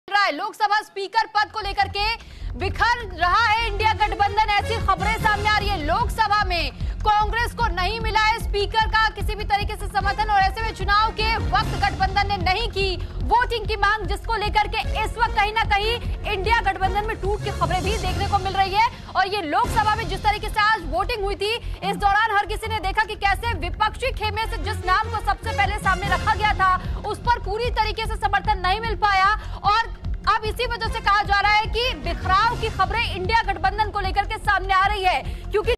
लोकसभा स्पीकर पद पर बिखरा इंडिया गठबंधन, कांग्रेस को नहीं मिला स्पीकर का साथ | Breaking News
Breaking News: लोकसभा स्पीकर पद पर बिखरा इंडिया गठबंधन, कांग्रेस को नहीं मिला स्पीकर का साथ